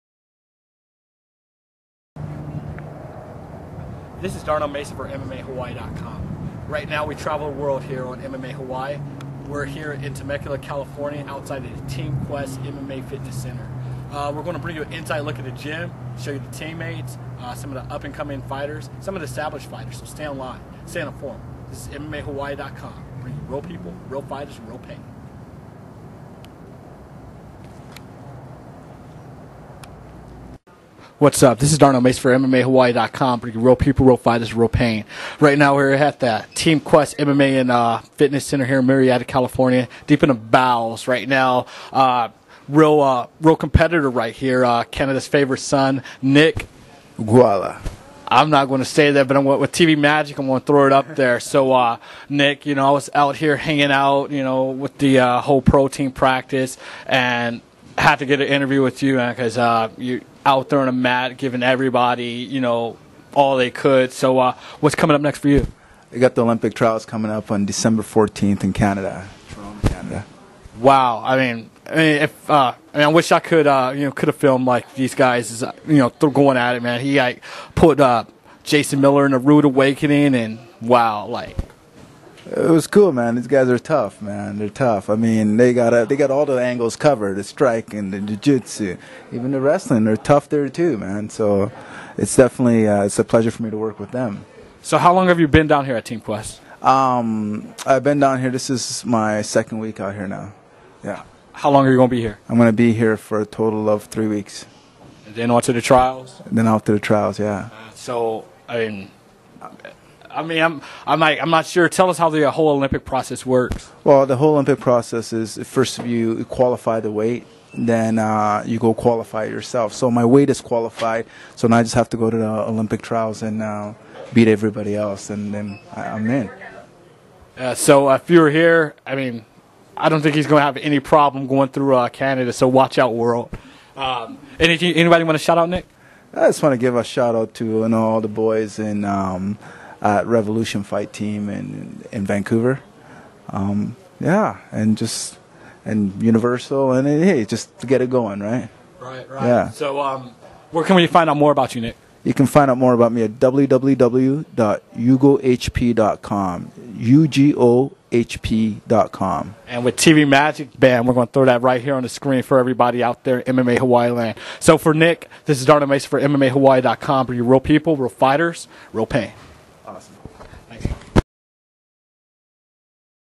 MMAHawaii.com Hanging with Nick Ugoalah
Nick Ugoalah still sweaty from the intense pro team practice at Team Quest discusses his Olympic and MMA dreams.